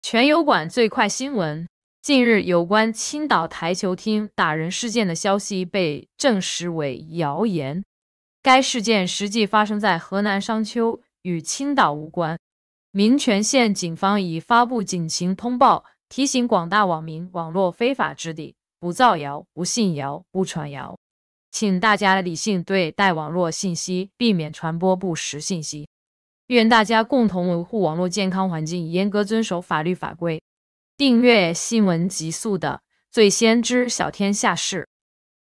中国新闻03月21日20时：辟谣：青岛某台球厅男子暴打女子后脑勺？ 假的！
近日，有关青岛台球厅打人事件的消息被证实为谣言。该事件实际发生在河南商丘，与青岛无关。民权县警方已发布警情通报，提醒广大网民网络非法之地，不造谣、不信谣、不传谣。请大家理性对待网络信息，避免传播不实信息。愿大家共同维护网络健康环境，严格遵守法律法规。